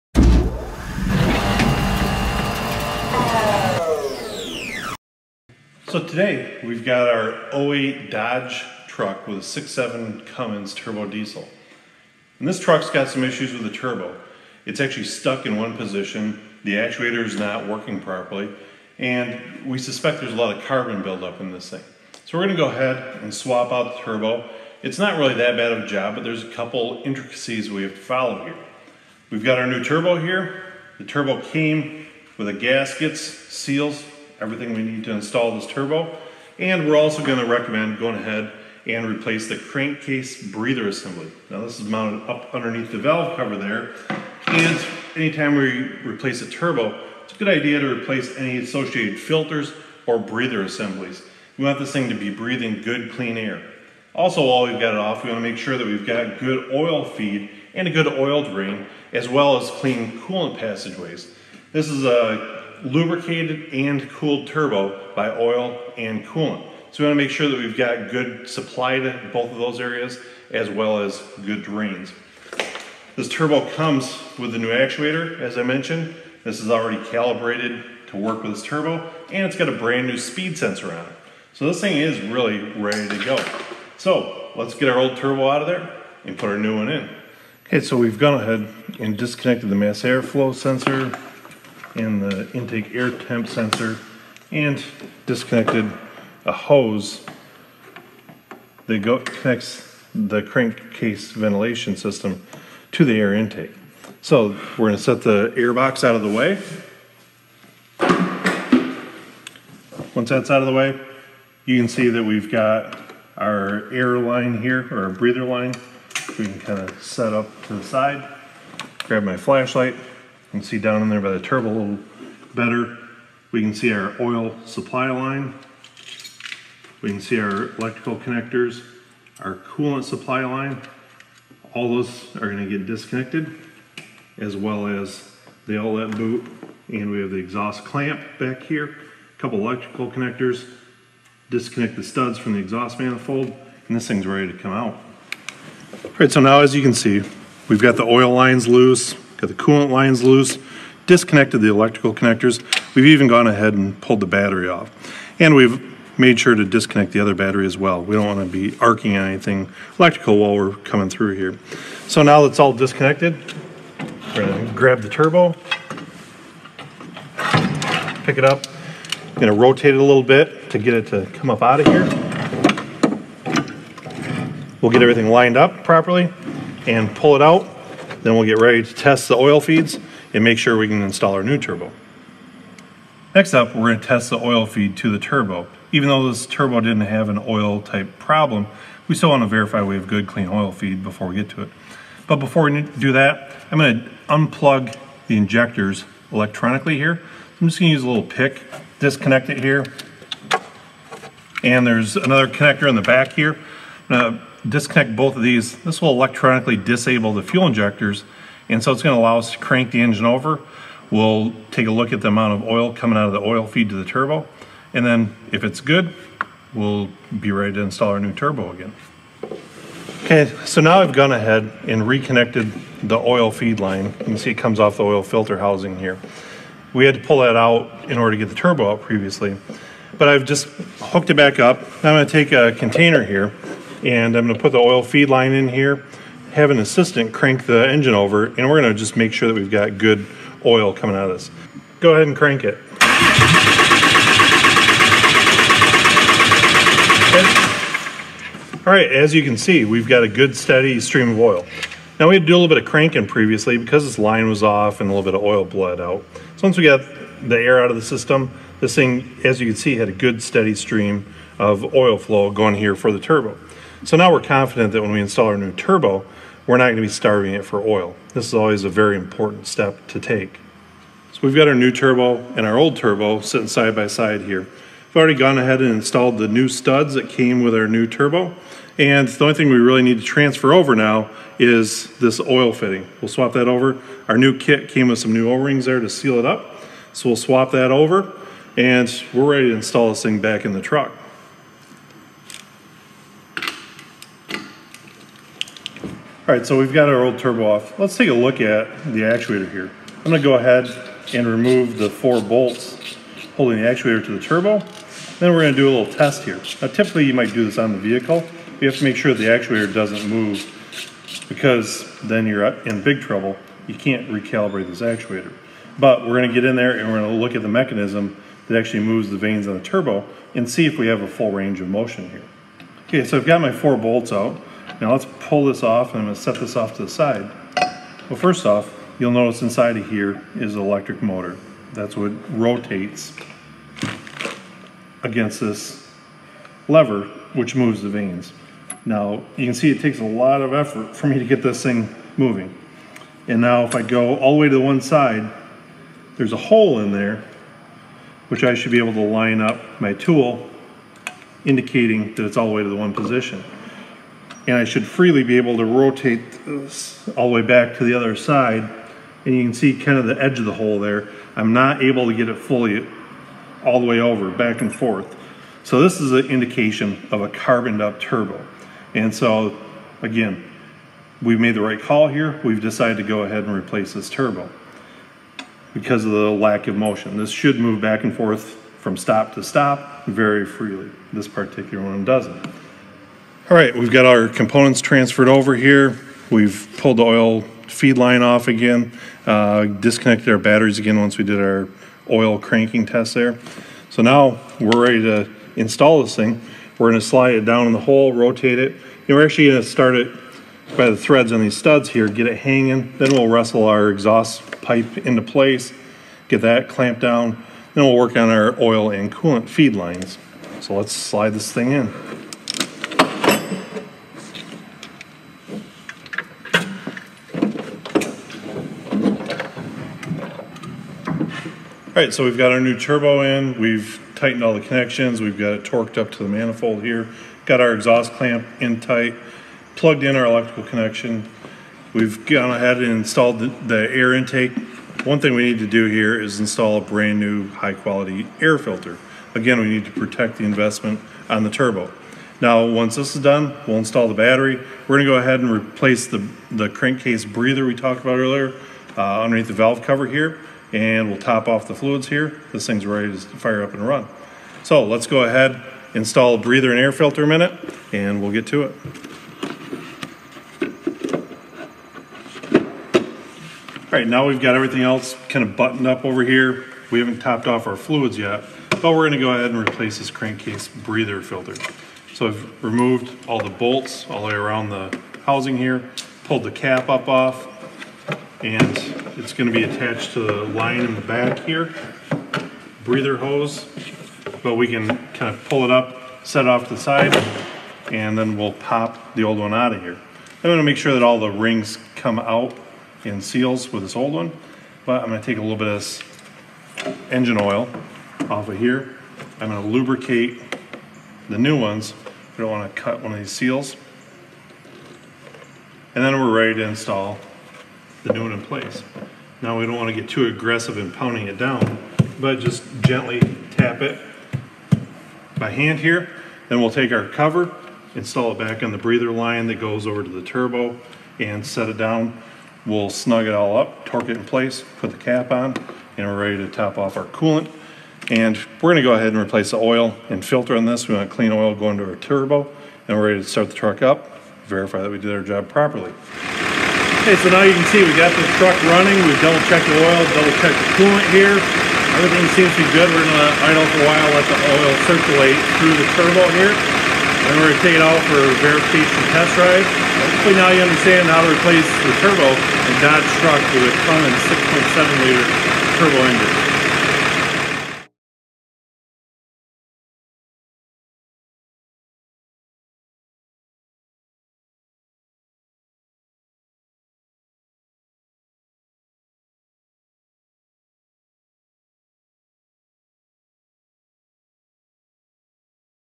Dodge 6.7 Cummins Turbo Replacement
Helpful tips to consider when replacing a turbocharger on a Dodge 6.7 Cummins engine. Here's what you need to know about the job:

Approximate installation time: 4 HOUR
Tools needed: 10mm, 13mm sockets, 3/8" ratchet, 13mm open end wrench, pliers, pick
Additional parts: Engine coolant, air filter, breather filter, engine oil

Vehicle used: 2008 Dodge Ram 2500